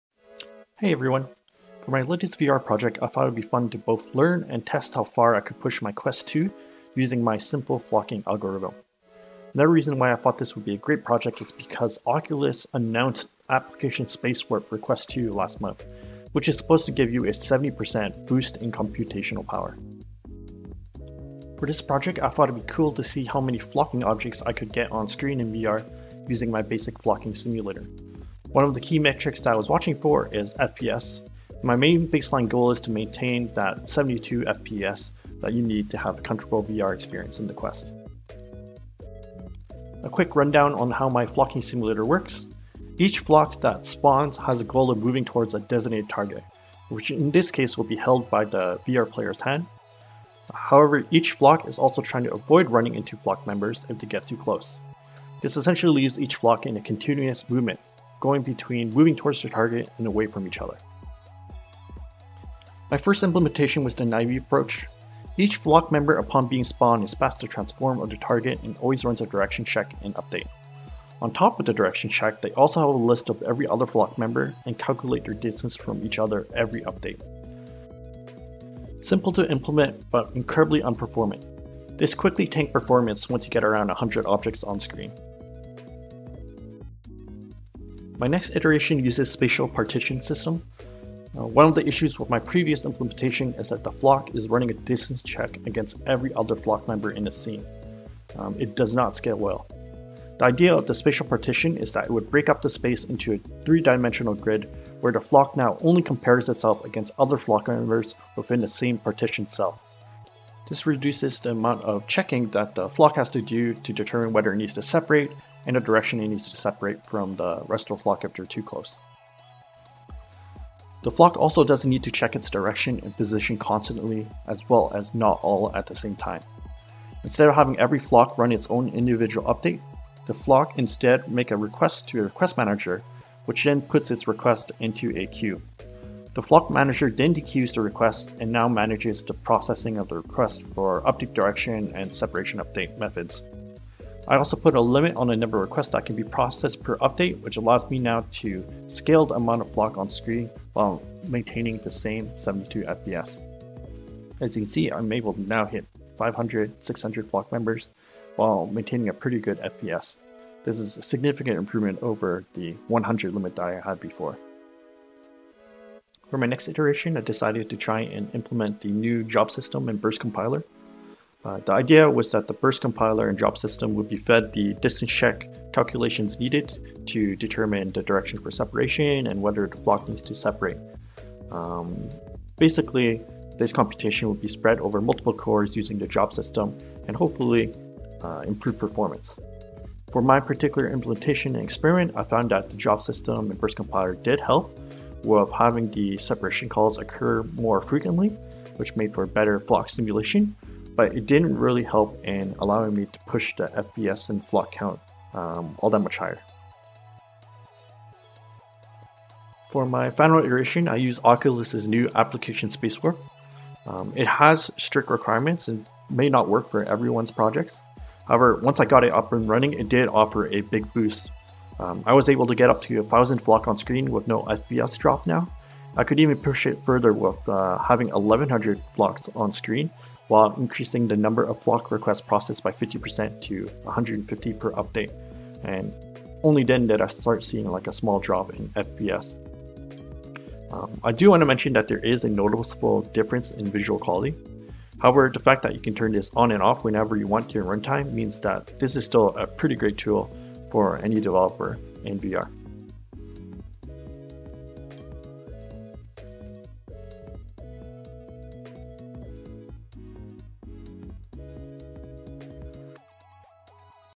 1000 Flock Object in Quest 2 - Testing Application Space Warp
My VR Test project to see how many flying objects I can have on the Quest 2 without impairing FPS. Tested out the new Oculus Application Space Warp for the Quest 2 as well.